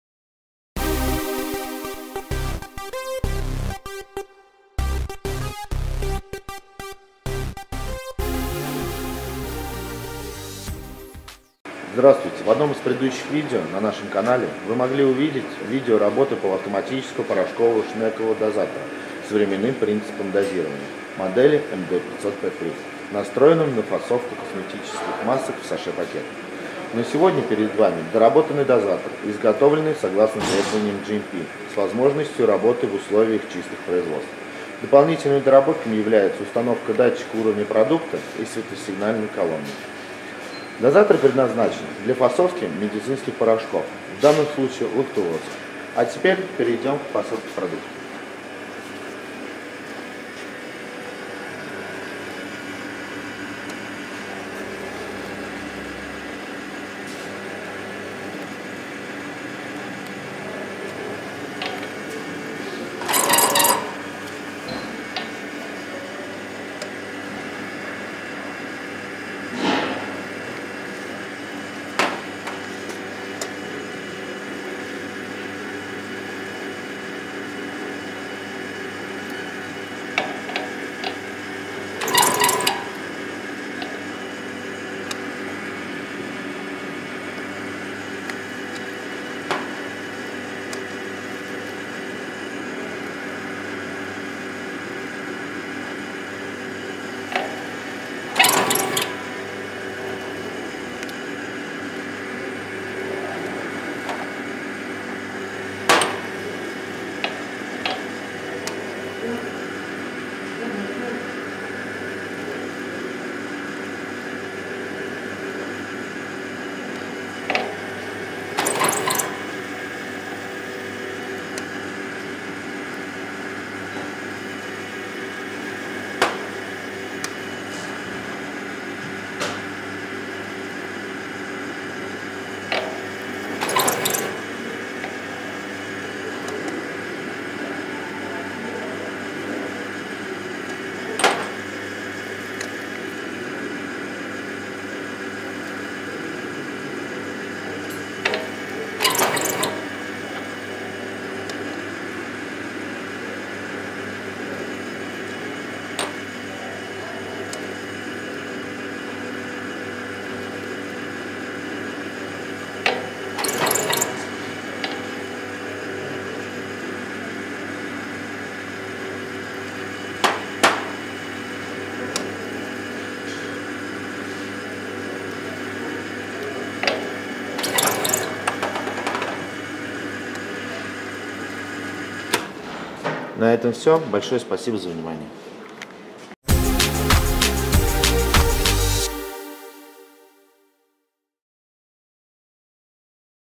Полуавтоматический дозатор для фасовки сыпучих продуктов, порошков МД-500П3.
В данном видео показано дозирование медицинского порошка во флаконы при помощи полуавтоматического дозатора МД-500П3.

Описание:

Полуавтоматический временной дозатор МД-500П3 предназначен для дозированной подачи порошкообразного продукта во флаконы, например, сухого молока, крахмала, порошковых пестицидов, косметических порошков, приправ, фармацевтических порошков и т.п. Изделие может применяться в косметической, пищевой и фармацевтической отраслях. Оборудование соответствует требованиям GMP. Все элементы, не контактирующие с продуктом, выполнены из нержавеющей стали 304L, а элементы, контактирующие с продуктом (бункер, шнек и ворошитель), из нержавеющей стали 316L, что препятствует коррозии. Порошковый дозатор оснащен системой PLC и LCD, драйвером и мотором-редуктором.
Модуль оснащен однопозиционным столом. Оператор вручную выставляет тару на стол. Дозатор приводится в рабочее состояние нажатием педали или кнопкой на панели управления. В зоне фасовки флаконы наполняются продуктом из сопла. После окончания процесса, оператор убирает наполненные флаконы и ставит новые, подготовленные к фасовке.
Высокая точность дозирования порошка достигается за счет использования прецизионного шагового привода шнека и его особой конструкции. Самопозиционирующийся шнек имеет три степени свободы. По сравнению с решениями, использующими жесткую конструкцию, у дозирующего шнека МД-500П3 отсутствует биение на конце. Шнек всегда находится по центру дозирующего сопла. Таким образом, нет выработки металла на той стороне внутренней поверхности сопла, к которой прижимается некорректно ориентированный шнек.
Полуавтоматический дозатор может работать в двух режимах: автоматическом и ручном. Если переключатель режима дозирования находится в позиции «автомат», то управление происходит по заданным параметрам. Если переключатель режима дозирования находится в позиции «ручной» режим, то управление происходит через нажатие педали.

С более подробным описанием данного оборудования Вы можете ознакомиться на нашем сайте по ссылке

Характеристики:

Количество дозирующих сопел: до 1 шт;
Мощность: до 1,5 кВт;
Вес: 55 кг;
Габариты, мм (Д х Ш х В): 550 х 460 х 750;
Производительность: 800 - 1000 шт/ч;
Напряжение: 220;
Тип дозатора: Шнековые дозаторы для густых и сыпучих продуктов;
Тип продукта: порошок;
Тип тары: Стеклянная тара, Пластиковая тара;
Отрасль: фармацевтика, ветеринария, химия;
Продолжительность переналадки: до 15 мин;